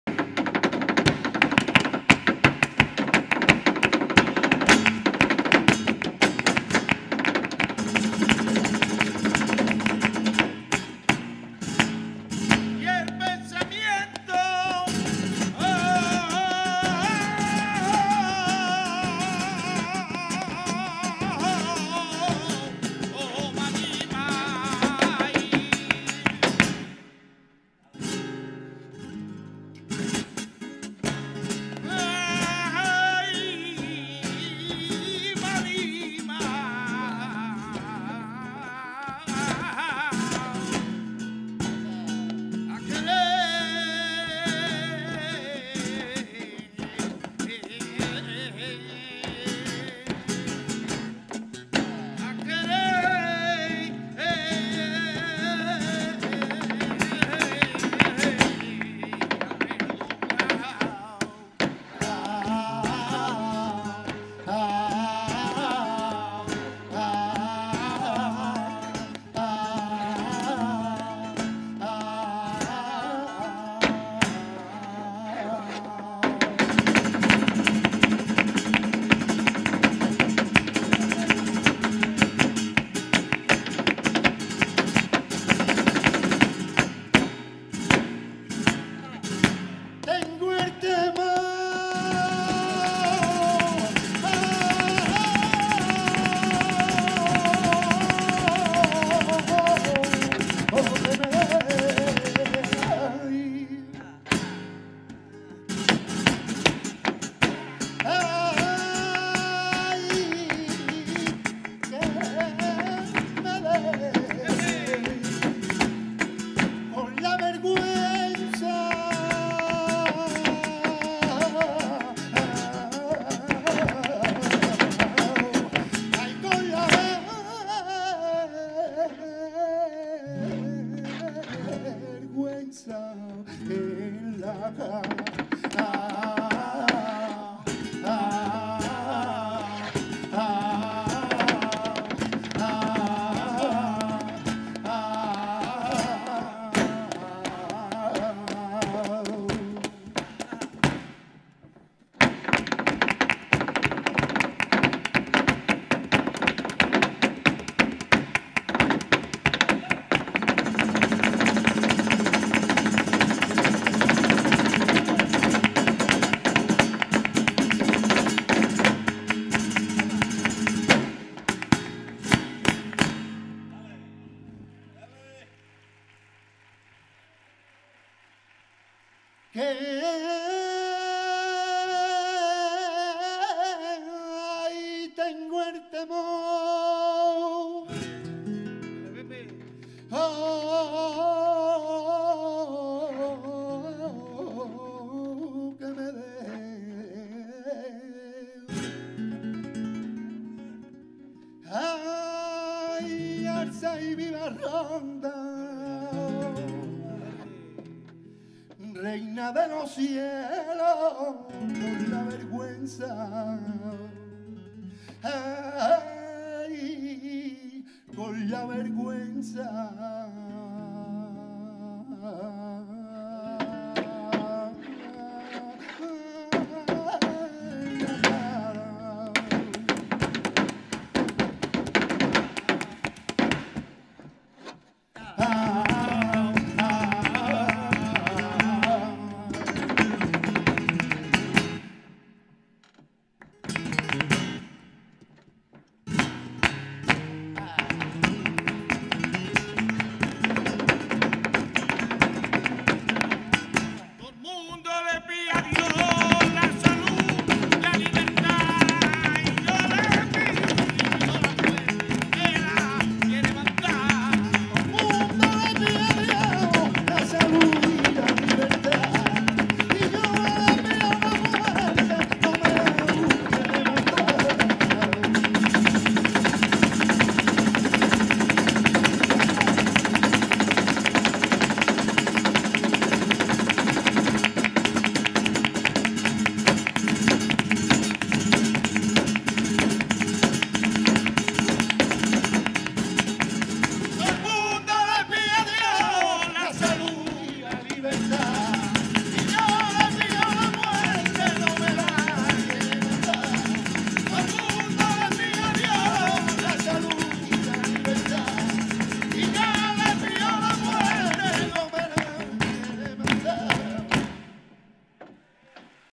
Rafael Ramírez Desplante Masculino 2021
El malagueño Rafael Ramírez baila por caña y se alza con el desplante masculino en el Festival Internacional de Cante de las Minas